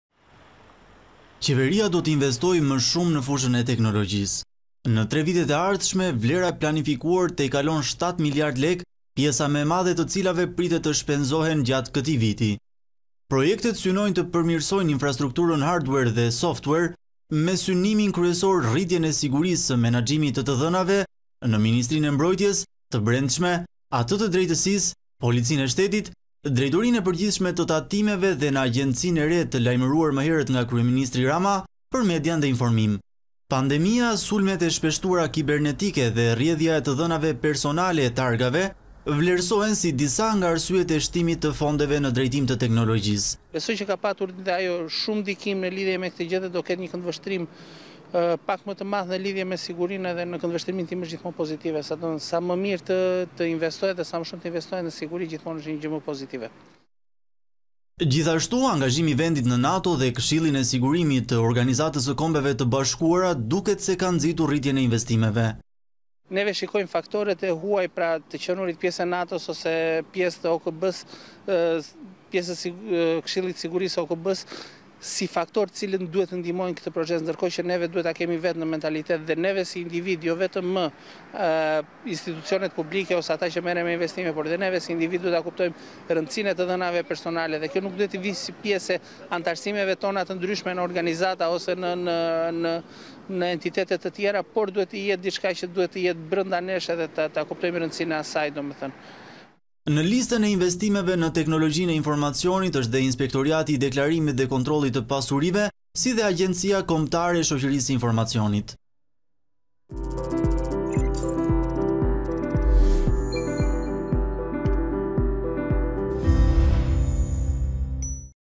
Rriten fondet për investimet në teknologji, synohet sigurimi i të dhënave
Pandemia, sulmet e shpeshtuara kibernetike dhe rrjedhja e të dhënave personale e targave kanë nxitur qeverinë të investojë më shumë në fushën e teknologjisë. Në tre vitet e ardhshme, vlera e planifikuar tejkalon 7 miliard lekë, pjesa më e madhe e të cilave pritet të shpenzohet gjatë këtij viti.